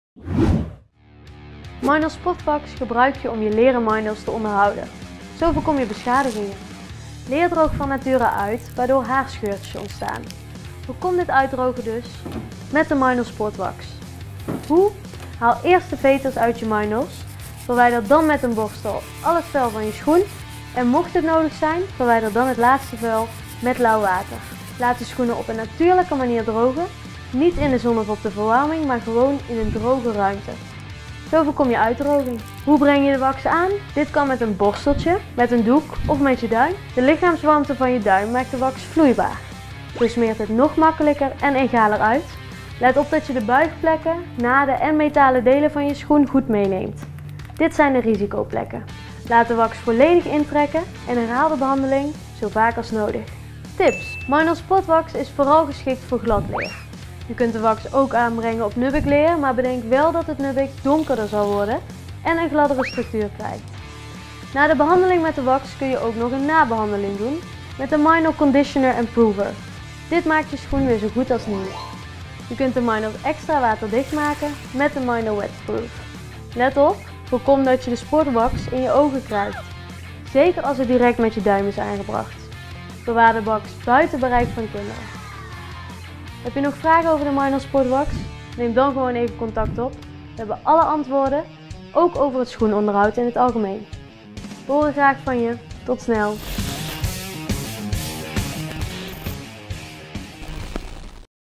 Met STACEY blijft het LEKKER VET 😋 | Meindl schoenonderhoud (werkschoenen) met Sport wax
Leer droogt van nature uit. Hierdoor ontstaan haarscheurtjes. Voorkom dit uitdrogen met de Meindl Sport Wax. Dit doe je door eerst je veters uit je Meindls te halen. Verwijder met een borstel al het vuil van je schoen en mocht het nodig zijn, verwijder dan het laatste vuil met lauw water. De schoenen laat je hierna op een natuurlijke manier drogen. Smeer je Meindl daarna in met de Sport Wax. Ten slot laat je de Wax volledig in trekken en herhaal je dit zo vaak als nodig.